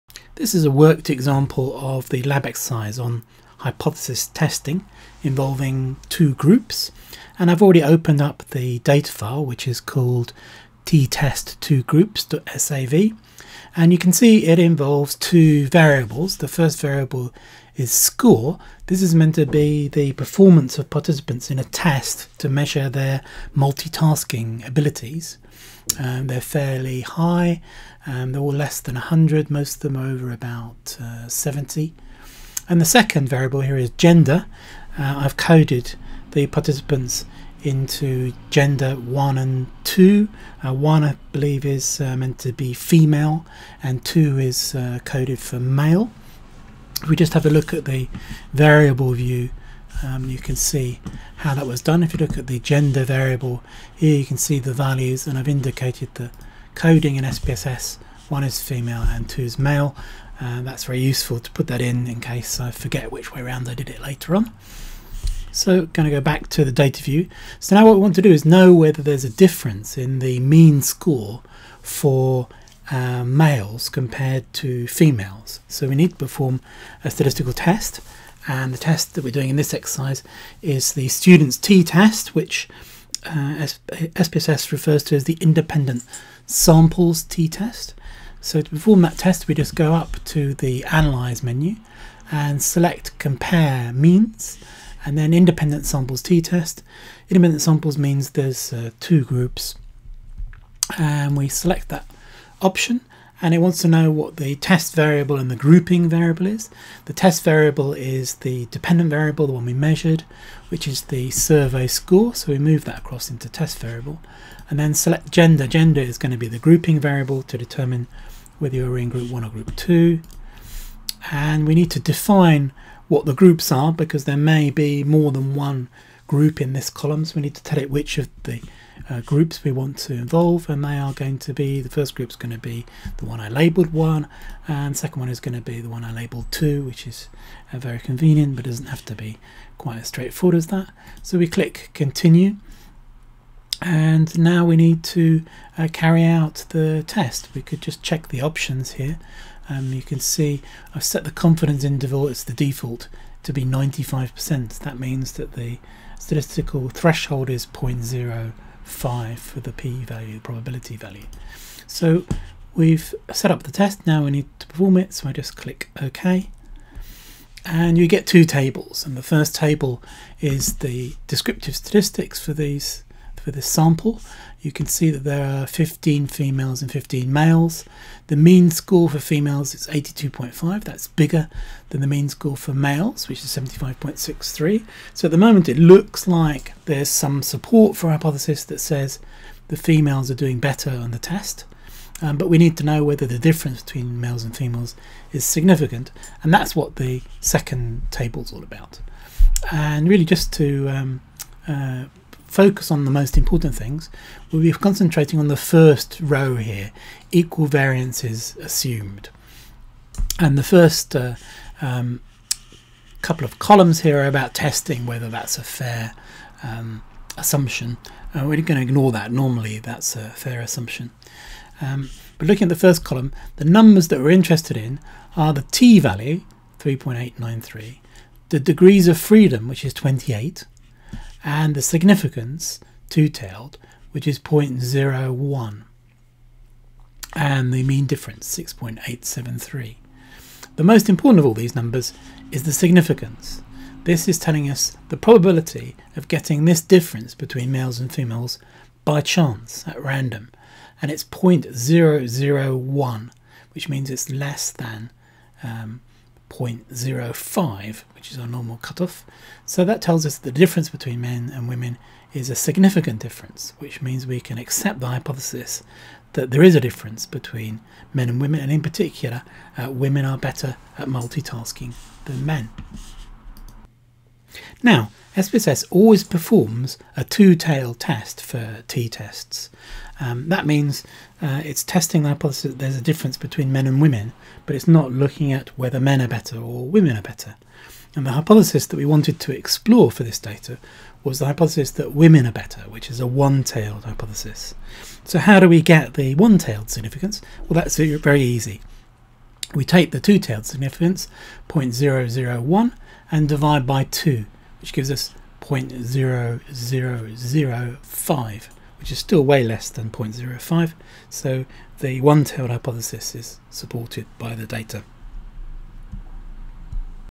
SPSS Student's t-Test (Two Groups)
How to perform a one-tailed Independent Samples t-Test in SPSS 20